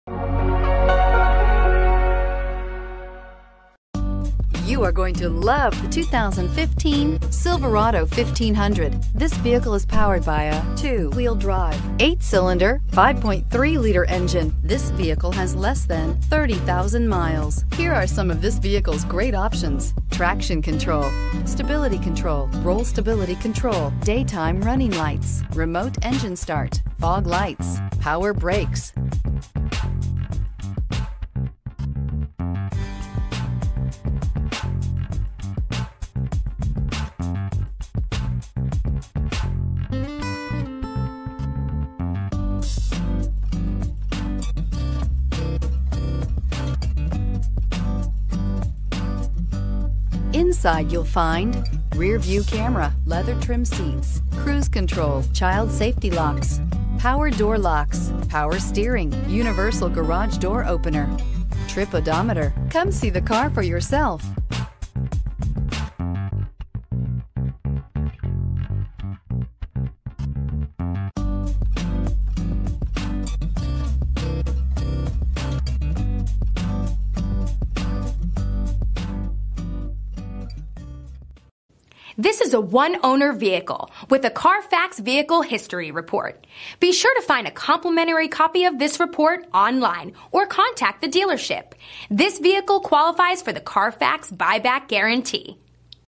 2015 Chevrolet Silverado 1500 LTZ for sale in Cary NC
2015 Chevrolet Silverado 1500 LTZ for sale in Cary NC - Stock # SB53405A
Year: 2015
Engine: 5.3L V8
Transmission: 6-Speed Automatic Electronic with Overdrive
Exterior Color: White Diamond Clearcoat
Interior Color: cocoa
Model Code: CC15543
Drive Line: RWD
Mileage: 28381
Address:
VIN: 3GCPCSEC7FG334860